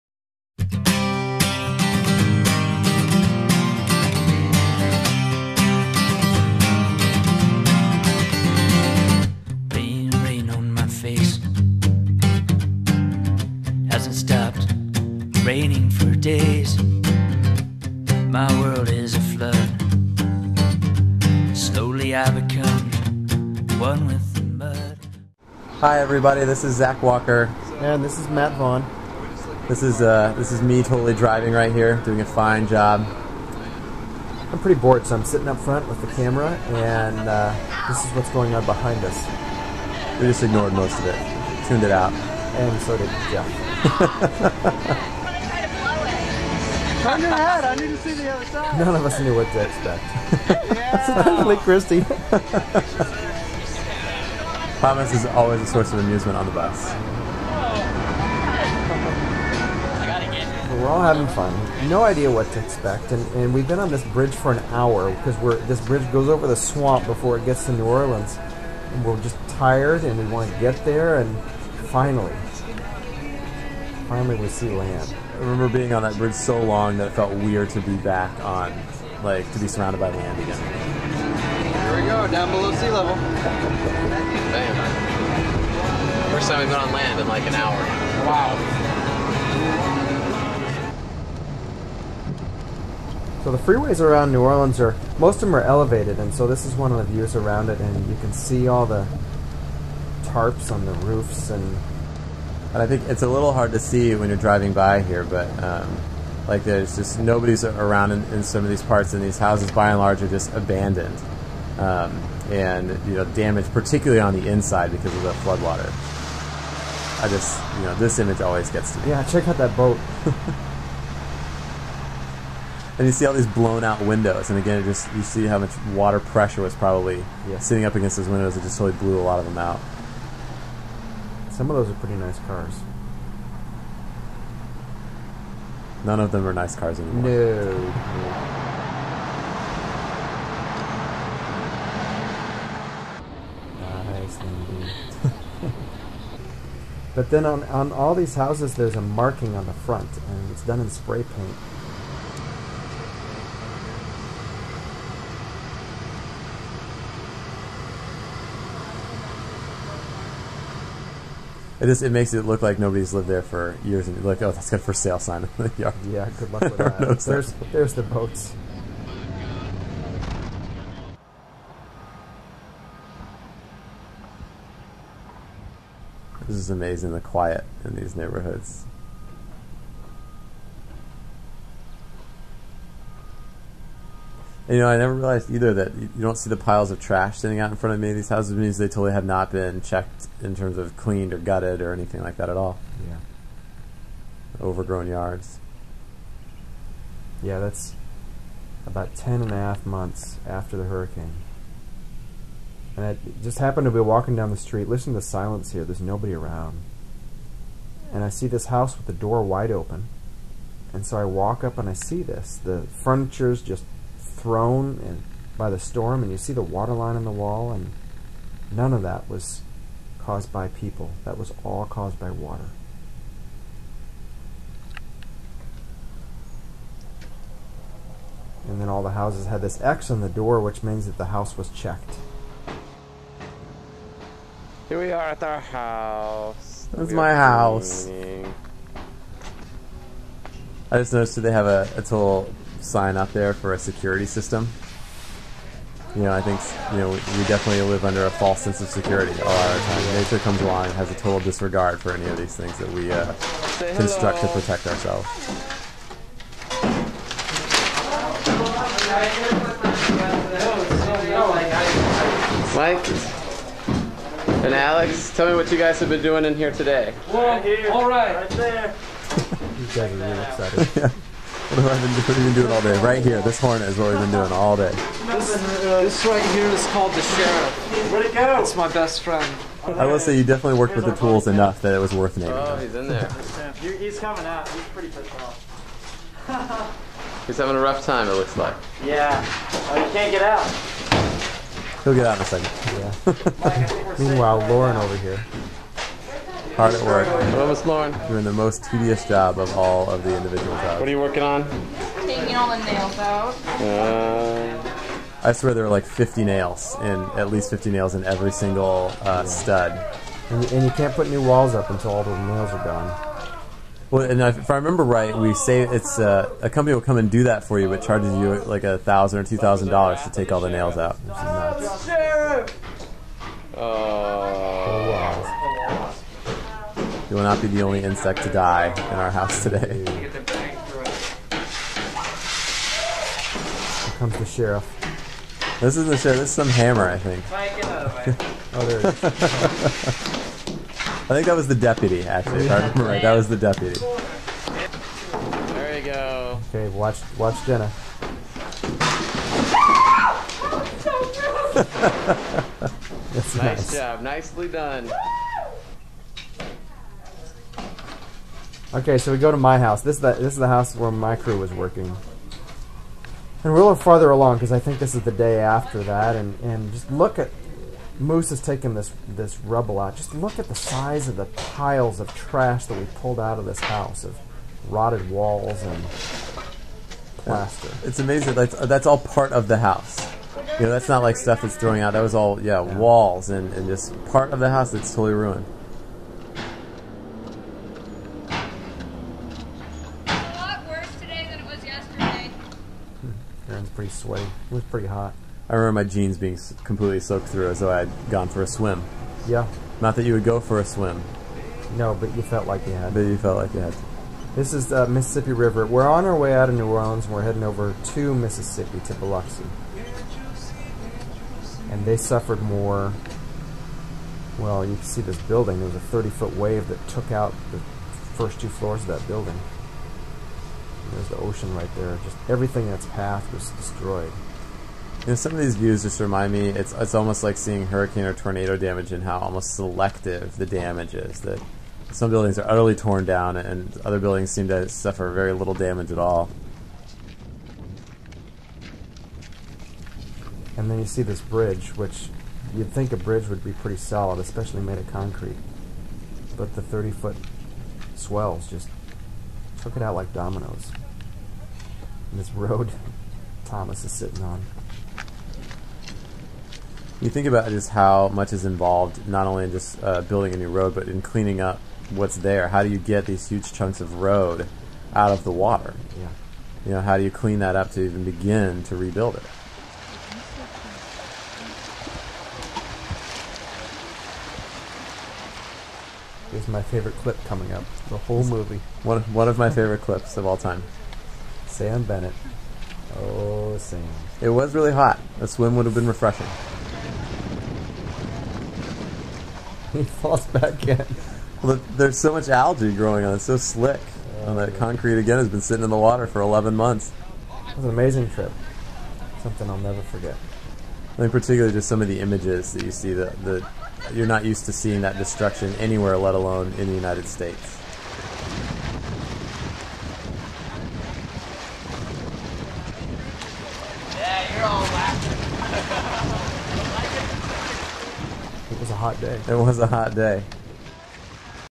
New Orleans 2006 Mission Trip
Village Presbyterian Church sent 43 high school youth and advisors to New Orleans in July, 2006, to help with the aftermath of Hurricane Katrina. Most of the work included gutting houses that had been affected by rising flood waters in 2005.